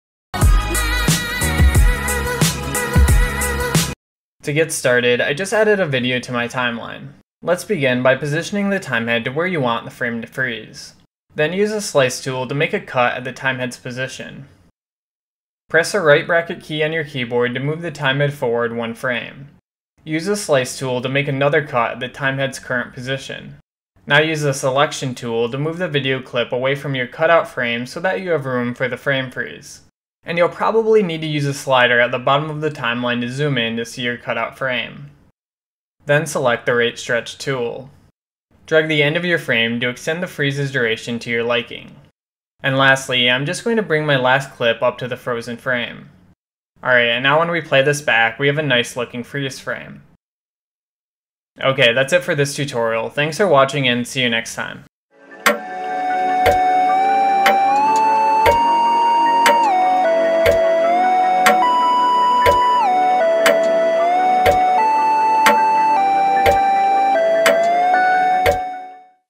Hitfilm Express : How to Freeze Frame
A tutorial on how to freeze frames in Hitfilm Express.  Frame Holds and pauses are useful in a lot of videos to stop time.